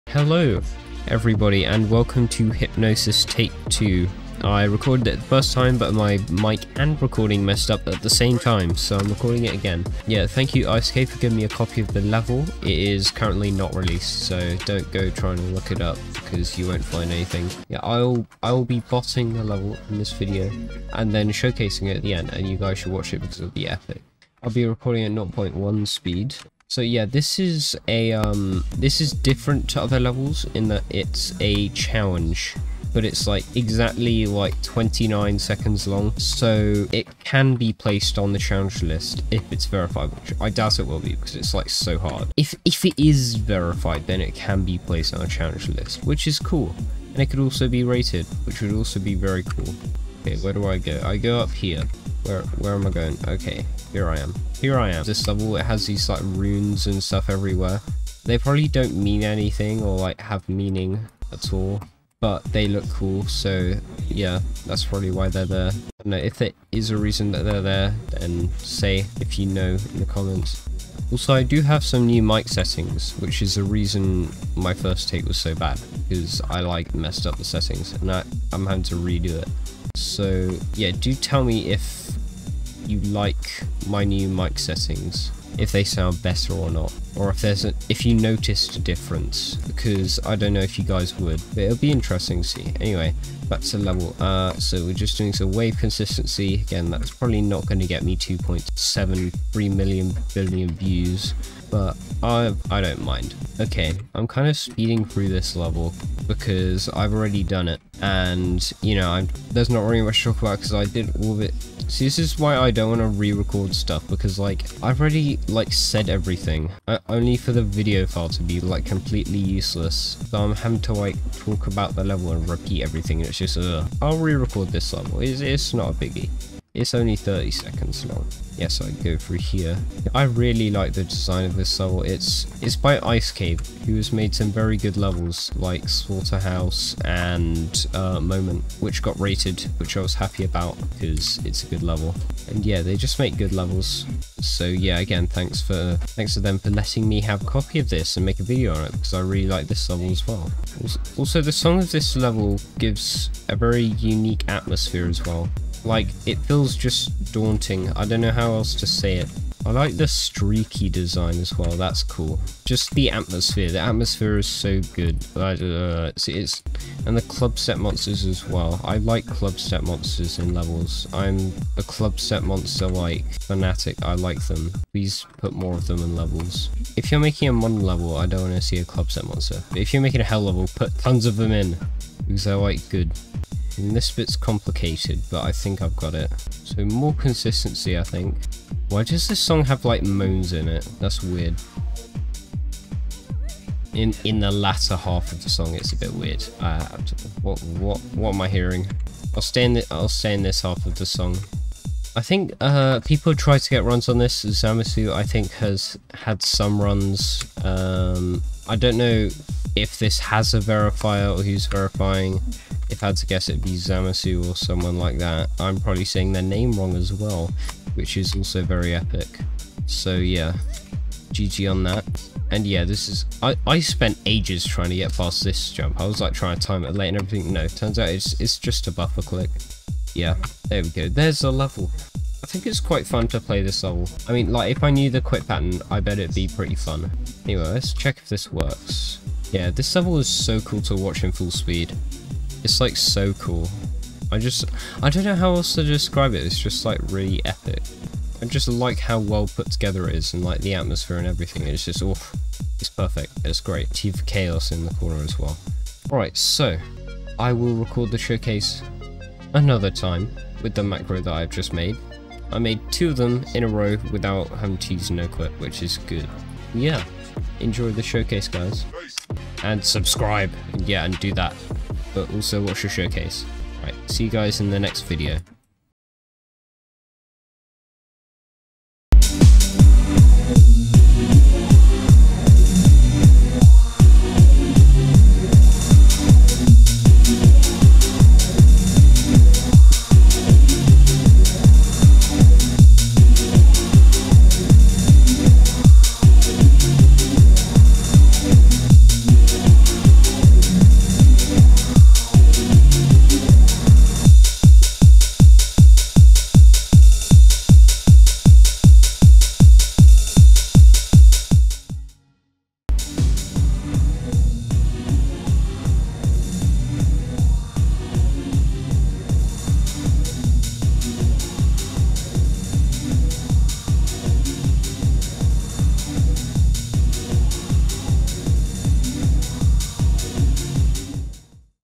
My FAVOURITE Challenge | Playing "Hypnosis" by icedcave
Thanks to icedcave for giving me a copy of the level to showcase + make a video on! I really like the level so I'm pretty glad I could finally get to play it myself. The level has a really cool atmosphere and overall is very daunting. The difficulty is very hard, kind of semi-impossible in the way slaughterhouse is, with zamasu achieving some small runs on the level here and there.